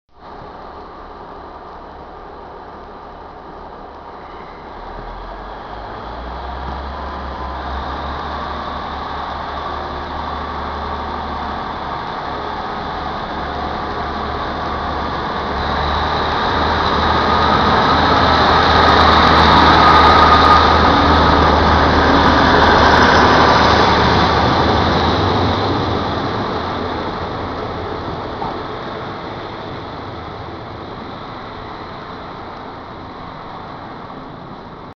150263 2M15 13/01/13 Westbury
150263 is seen leaving Westbury on the 13/01/13 with the 2M15 17:10 Westbury-Swindon FGW service.